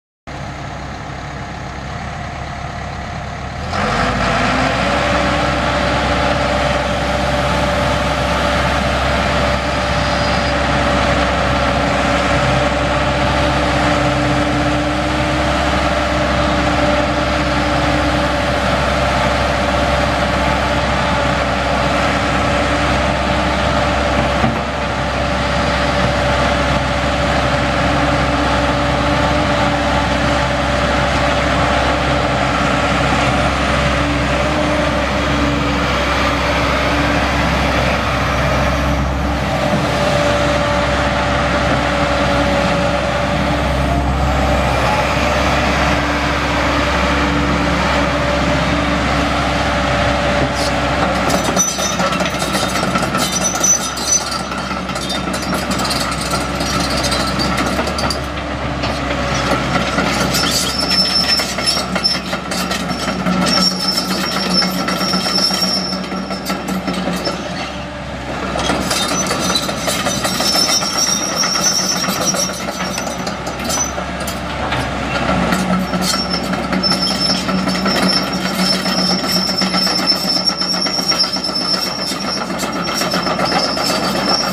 2016 KOBELCO SK210 LC-9 For Sale
4 CYL TURBO DIESEL, CAB WITH HEAT, A/C, STEREO, AUTO IDLE, 2 SPEED TRAVEL, 32" TRACK PADS, AUX HYD, 9' 8" STICK, BUCKET WITH TEETH, METER READS 3959 HOURS, S/N YQ13-T2459